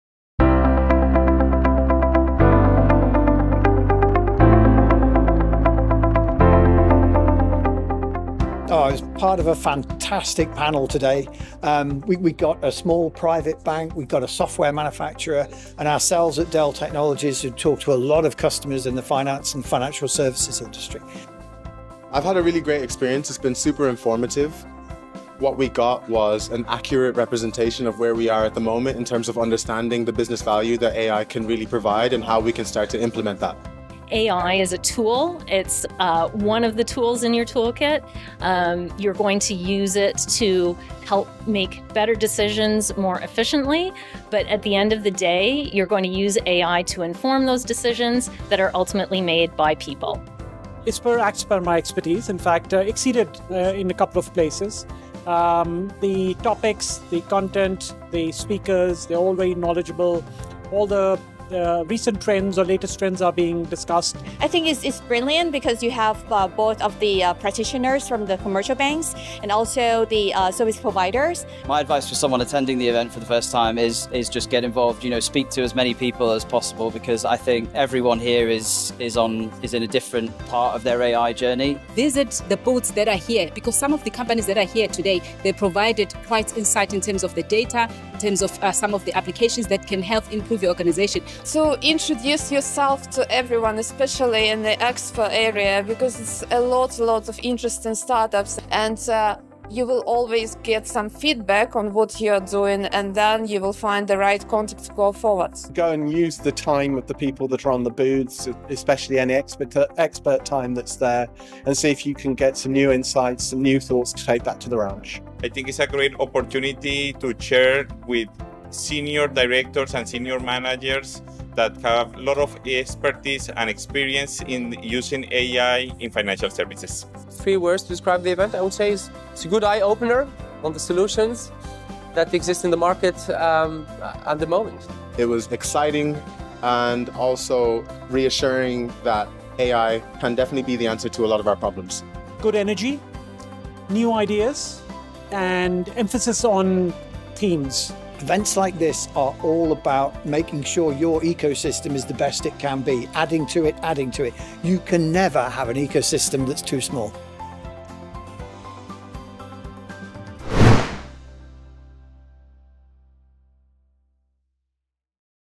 Artificial Intelligence in Financial Services Conference 2024
See highlights of our 2024 Artificial Intelligence in Financial Services Conference. Hear what our delegates, speakers and sponsors had to say about this must attend event.